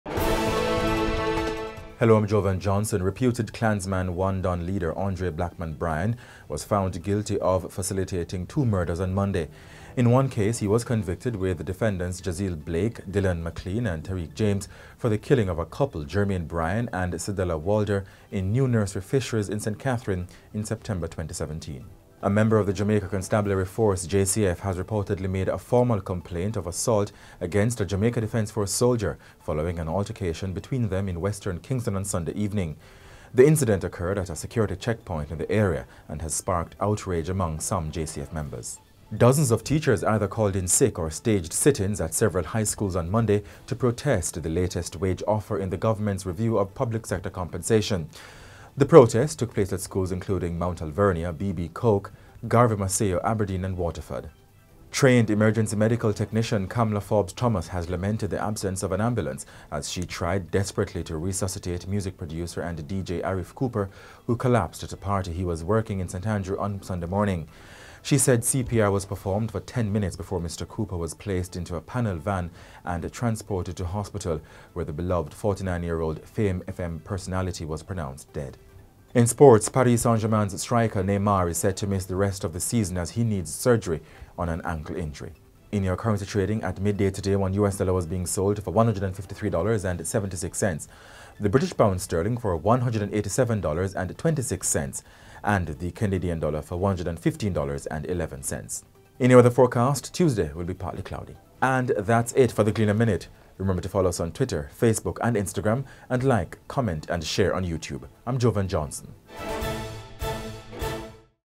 THE GLEANER MINUTE: Teachers’ protest | Cop accuses soldier of assault | Arif Cooper dies
MARCH 6, 2023: Reputed Clansman One-Don leader Andre Blackman Bryan was found guilty of facilitating the two murders on Monday…A member of the Jamaica Constabulary Force (JCF) has reportedly made a formal complaint of assault against a Jamaica Defense Force soldier, following an altercation between them in wester Kingston on Sunday evening... Dozens of teachers either called in 'sick' or staged sit-ins at several high schools Monday to protest the latest wage offer in the government’s review of public sector compensation...Trained emergency medical technician Kamla Forbes-Thomas has lamented the absence of an ambulance as she tried desperately to resuscitate music producer and DJ Arif Cooper who collapsed at a party he was working in St Andrew on Sunday morning… IN SPORTS: Paris St-Germain striker Neymar is set to miss the rest of the season as he needs surgery on an ankle injury...